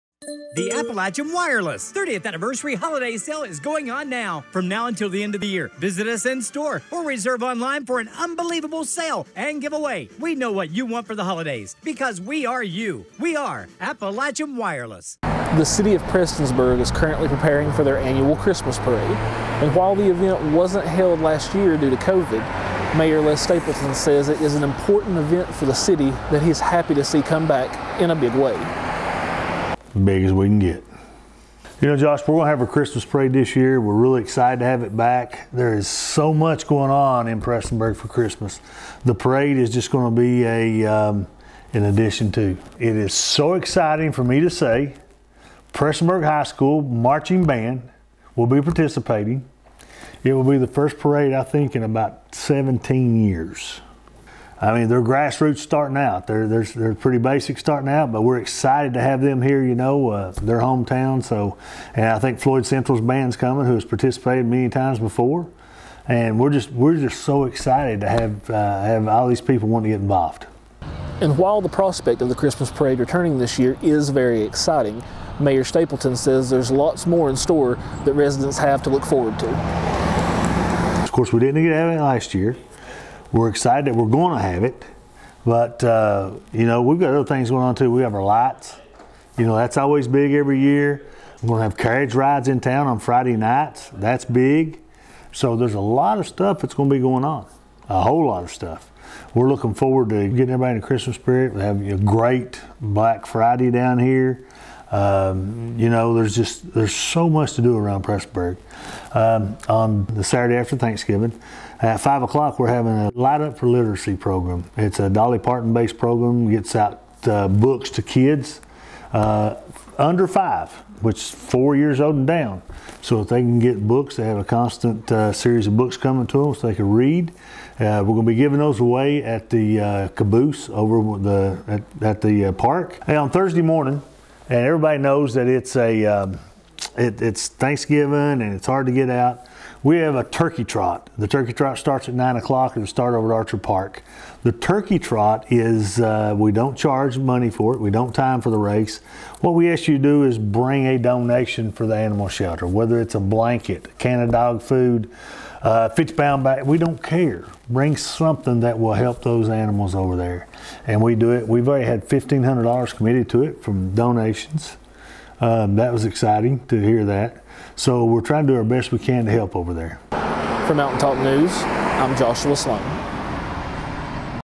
Prestonsburg Prepares for Christmas Parade and More
The City of Prestonsburg is preparing to host their annual Christmas Parade and many more activities this holiday season.